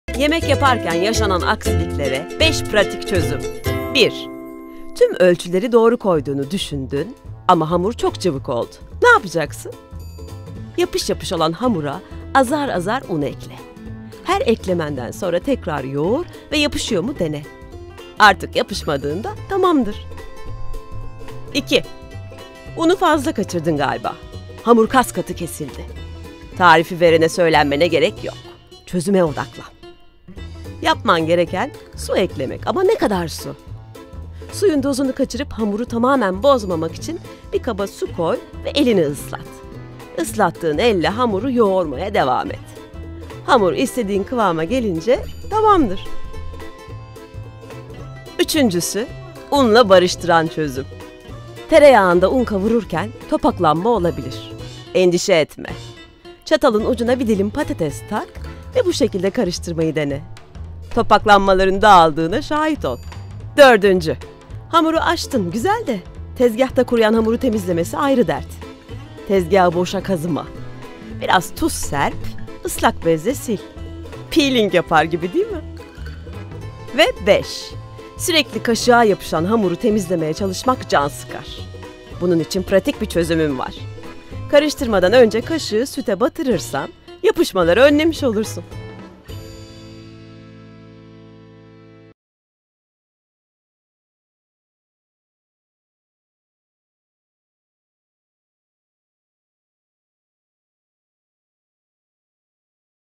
Yemek Yaparken Yaşanan Aksiliklere 5 Pratik Çözüm
Sarımsak nasıl soyulur? Sarımsağı daha kolay ve hızlı bir şekilde nasıl soyabilirsiniz? Sarımsağın aroması doğrama şekline göre nasıl değişkenlik gösterir? Sarımsak ile ilgili videomuzu izleyin.

BÖLÜMLER

00:05 Cıvık Hamur
00:23 Sert Hamur
00:50 Topaklanan Un
01:05 Tezgahta Kuruyan Hamur
01:18 Kaşığa Yapışan Hamur